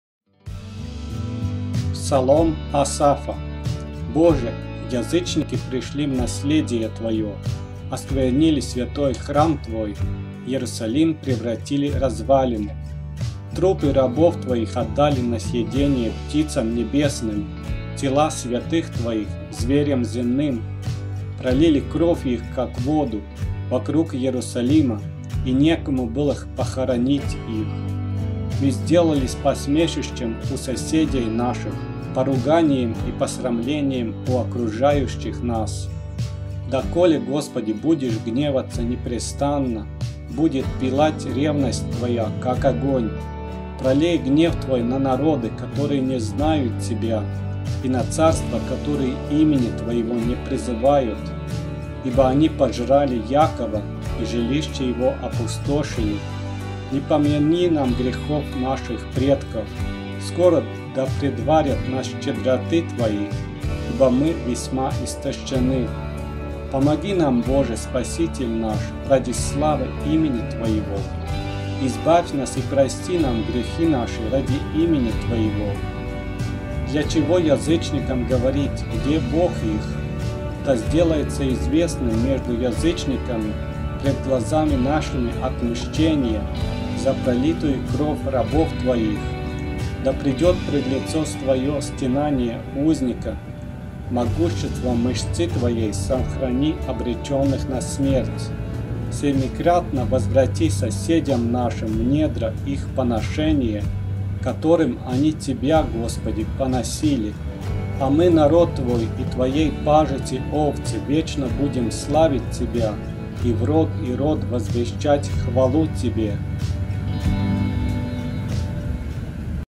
Псалом 78
1. Боже! язычники пришли в наследие Твое, осквернили святой храм Твой, Иерусалим превратили в развалины;
2. трупы рабов Твоих отдали на съедение птицам небесным, тела святых Твоих — зверям земным;
3. пролили кровь их, как воду, вокруг Иерусалима, и некому было похоронить их.
4. Мы сделались посмешищем у соседей наших, поруганием и посрамлением у окружающих нас.
5. Доколе, Господи, будешь гневаться непрестанно, будет пылать ревность Твоя, как огонь?
6. Пролей гнев Твой на народы, которые не знают Тебя, и на царства, которые имени Твоего не призывают,
7. ибо они пожрали Иакова и жилище его опустошили.
8. Не помяни нам грехов наших предков; скоро да предварят нас щедроты Твои, ибо мы весьма истощены.
9. Помоги нам, Боже, Спаситель наш, ради славы имени Твоего; избавь нас и прости нам грехи наши ради имени Твоего.
10. Для чего язычникам говорить: «где Бог их?» Да сделается известным между язычниками пред глазами нашими отмщение за пролитую кровь рабов Твоих.
11. Да придет пред лицо Твое стенание узника; могуществом мышцы Твоей сохрани обреченных на смерть.
12. Семикратно возврати соседям нашим в недро их поношение, которым они Тебя, Господи, поносили.
13. А мы, народ Твой и Твоей пажити овцы, вечно будем славить Тебя и в род и род возвещать хвалу Тебе.
(Псалтирь 78:1-13)